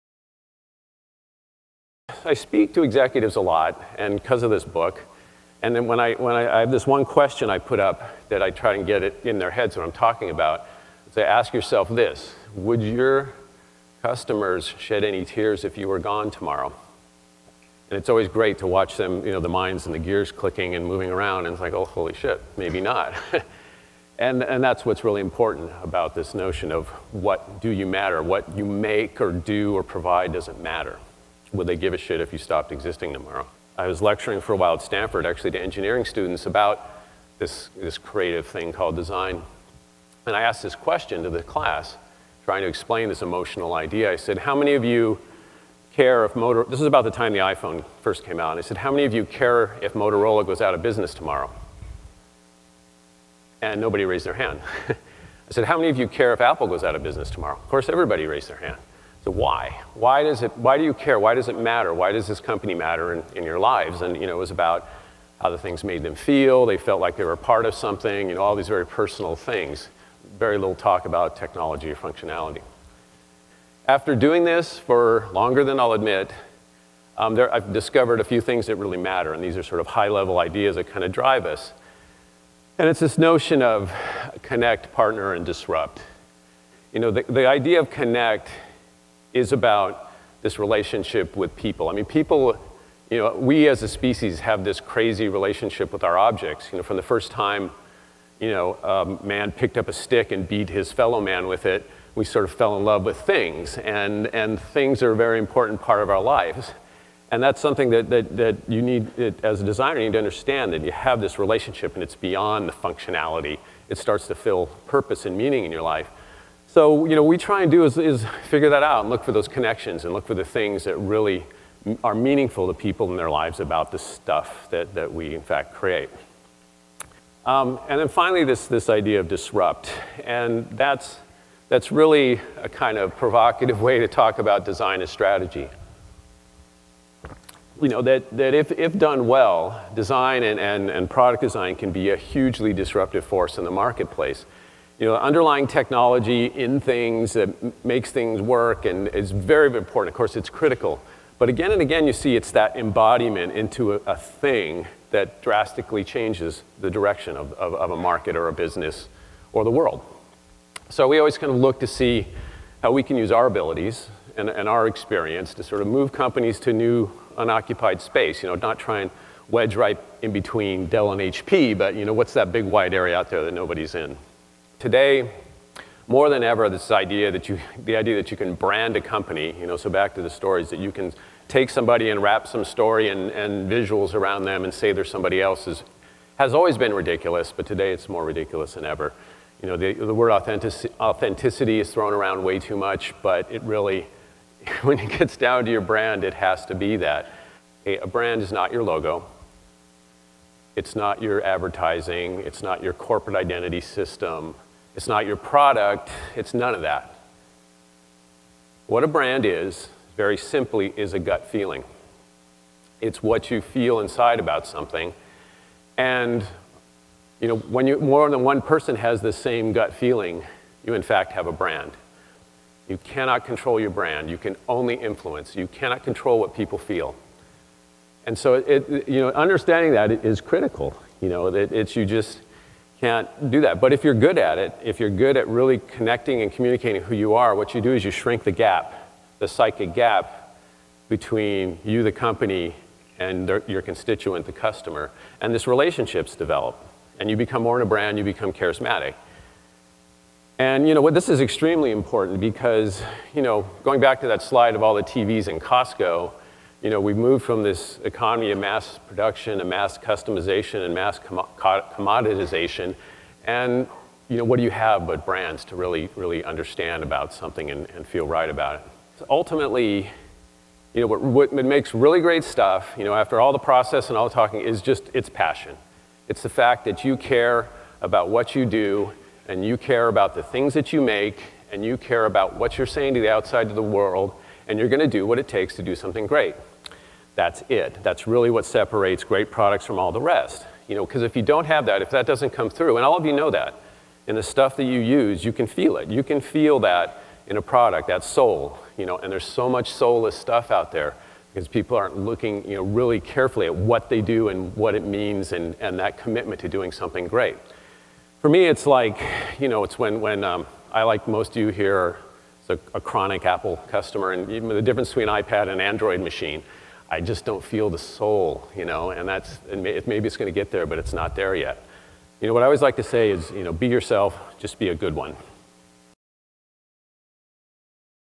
Designing Ideas, Not Objects | Interactive 2011 | SXSW
More that ever, the design of things is driving change into markets, and defining the idea of what a company means to its constitueants.  Today, you can't just create something, make and deliver it, and then expect it do well.  Products must be more than just a "thing". They need to built off relevant ideas, and the story needs to be driven throughout the entire proposition in an authentic way.

Though observations and work examples, designer Robert Brunner will talk about why the notion of "ideas, not objects" is so important now, and how he and his company have created products that embody this principle.  Highlighted will be his work for Barnes and Noble, Polaroid, Fuego, and Beats by Dr. Dre.